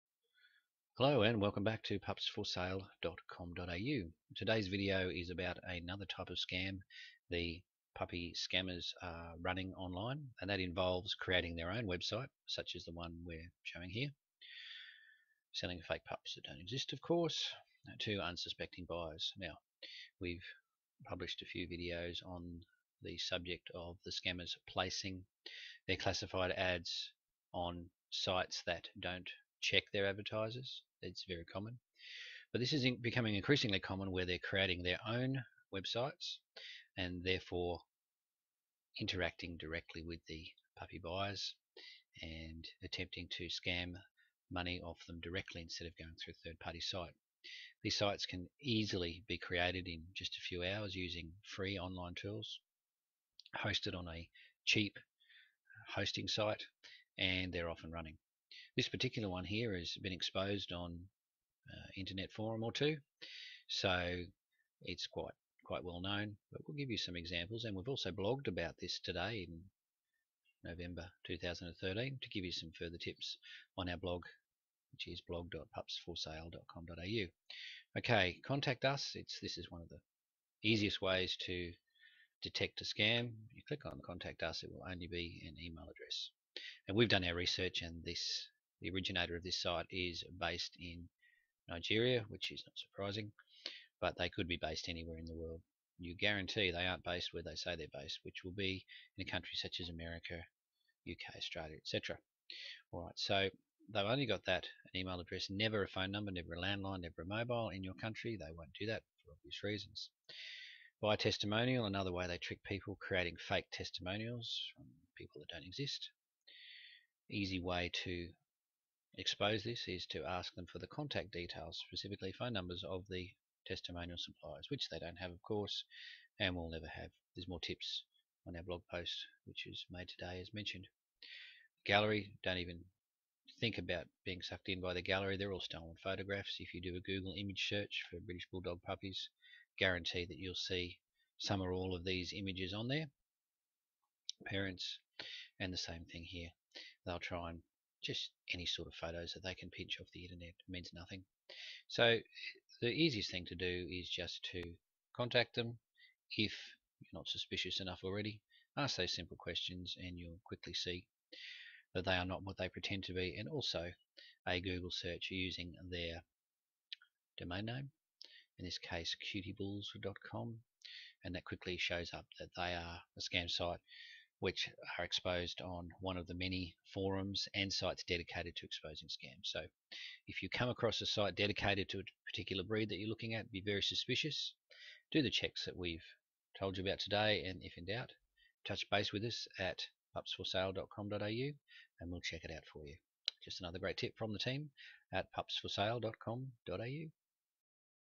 cutiebulls british bulldog puppies for sale scam
puppies for sale scams being run online are again exposed by the team at pups4sale. The site in question - cutiebulls.com - is just one example of the many fake sites on the internet selling non-existent puppies to unsuspecting puppy buyers.